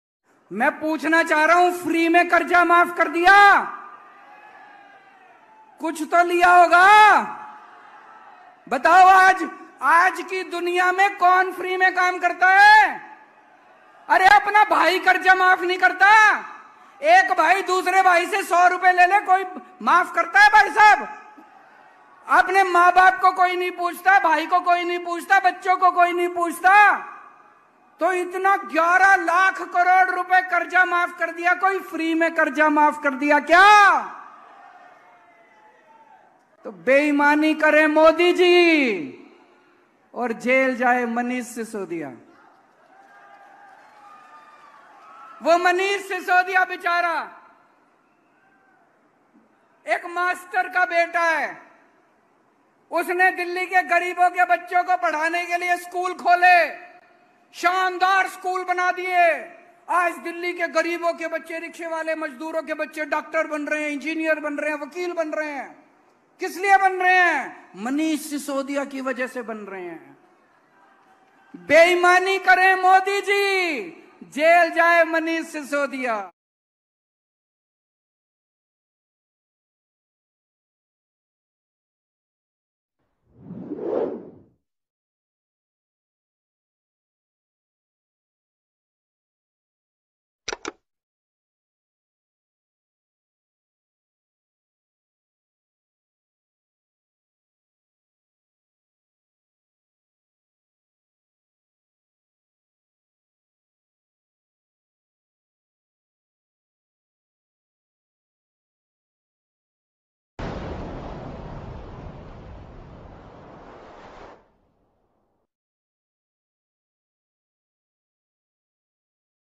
Arvind Kejriwal | '11 लाख करोड़ का कर्ज़ माफ़ किया, कुछ तो लिया ही होगा?' | AAP | Hindi News
'11 लाख करोड़ का कर्ज़ माफ़ किया, कुछ तो लिया ही होगा?'

सीएम अरविंद केजरीवाल का बीजेपी पर बड़ा आरोप

Source :- Twitter